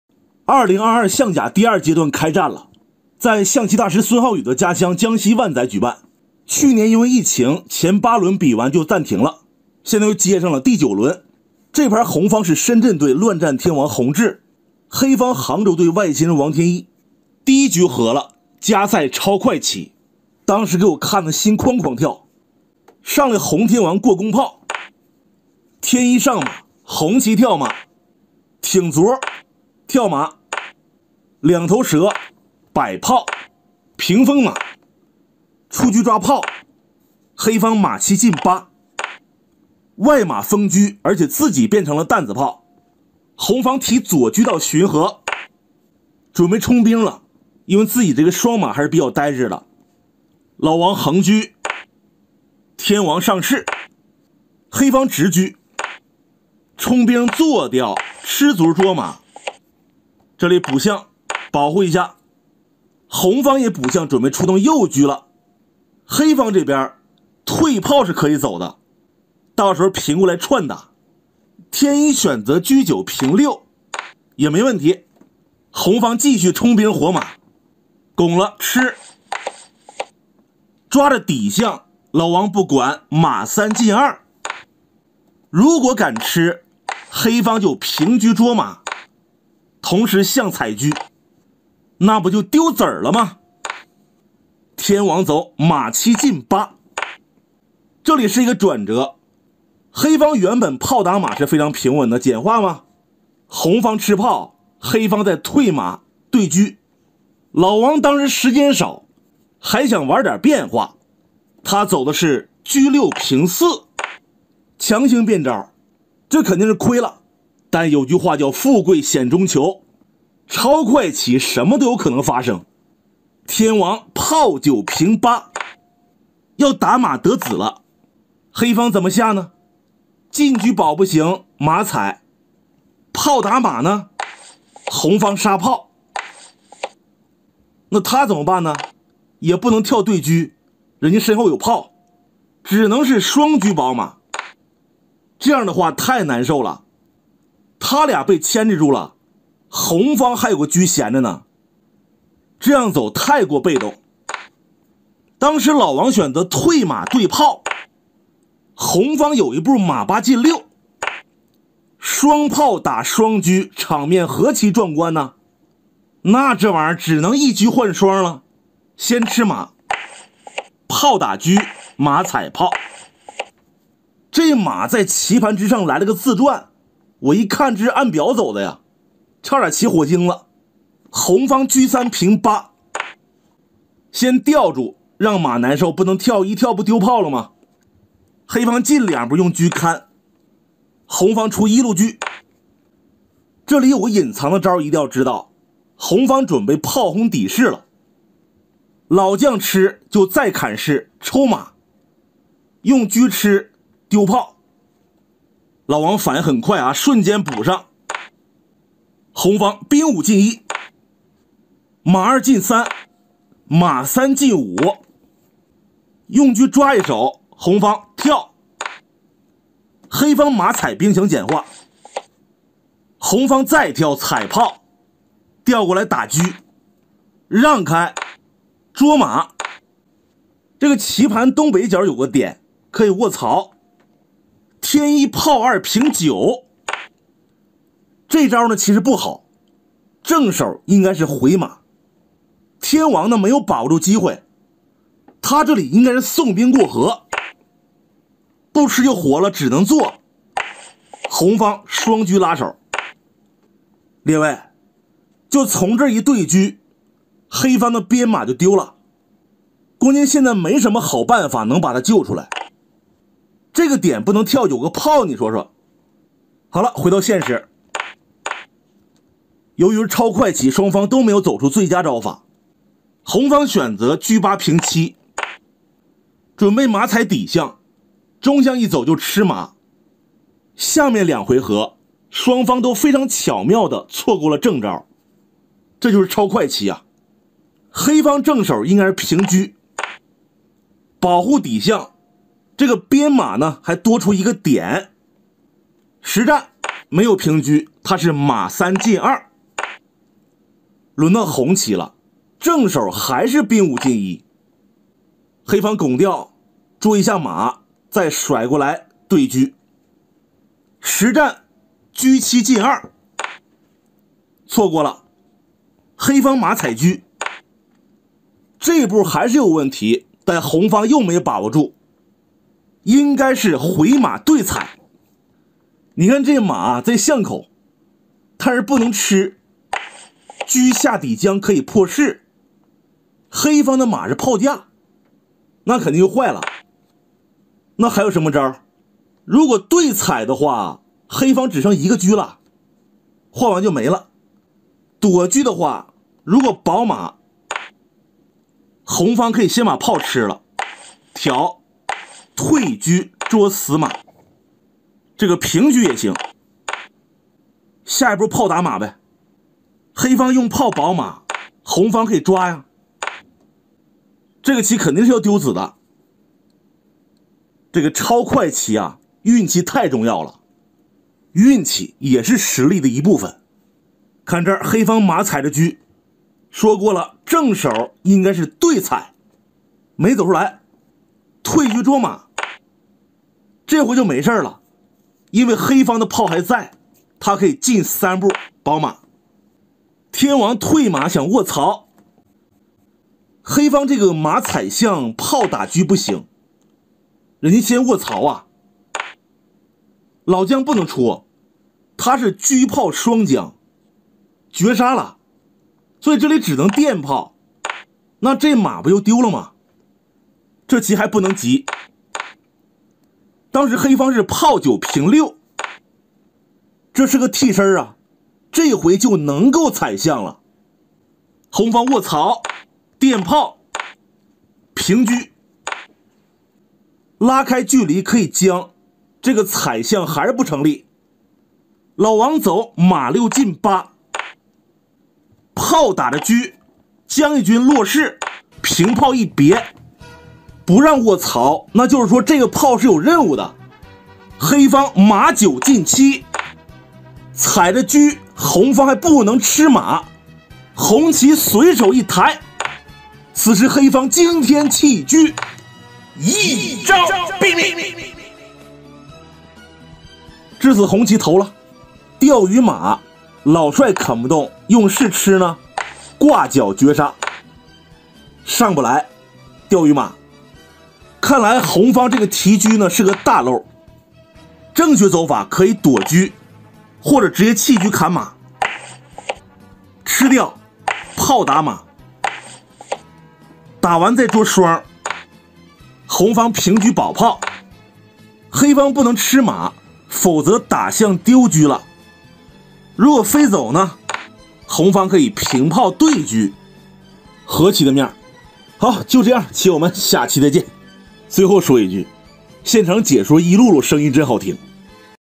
洪智vs王天一 超快棋特级大师都蒙了 弃车连杀 2022象甲【四郎讲棋】
洪智vs王天一 超快棋特级大师都蒙了 弃车连杀 2022象甲！